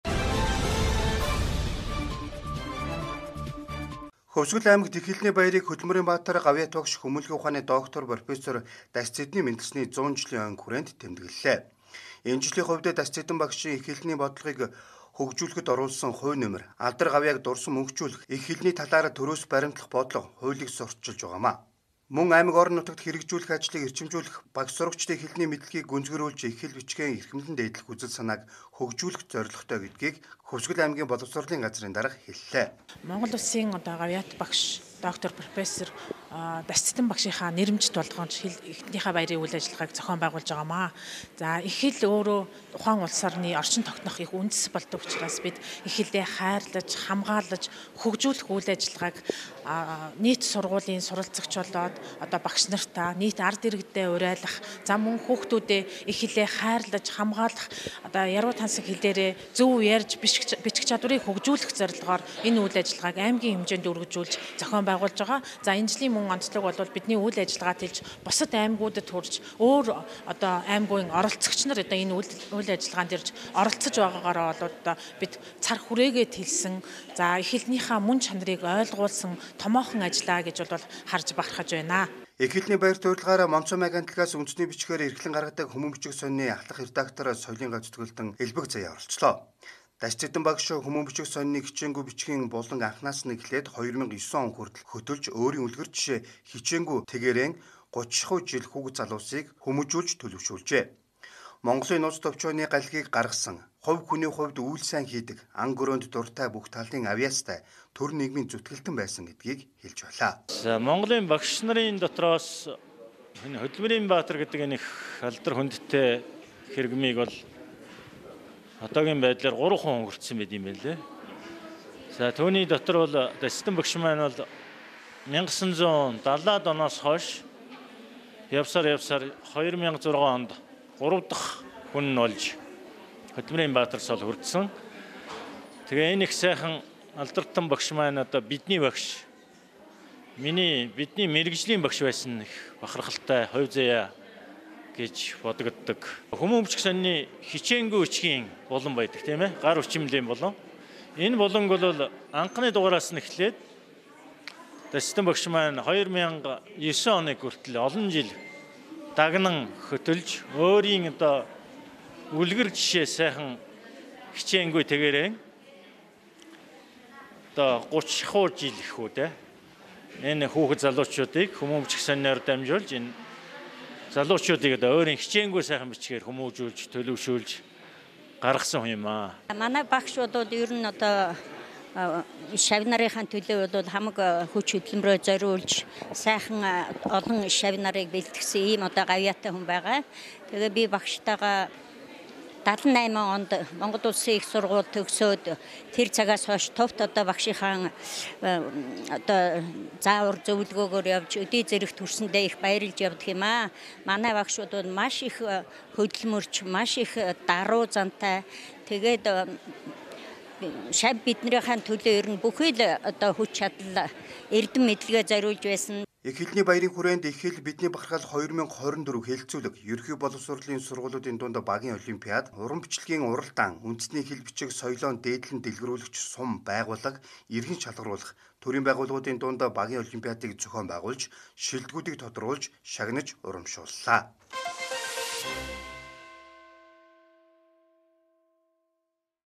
Эх хэлний баярыг Хөдөлмөрийн баатар Т.Дашцэдэний мэндэлсний 100 жилийн ойн хүрээнд тэмдэглэж байна
ХӨВСГӨЛ: Эх хэлний баярыг Хөдөлмөрийн баатар Т.Дашцэдэний мэндэлсний 100 жилийн ойн хүрээнд тэмдэглэж байна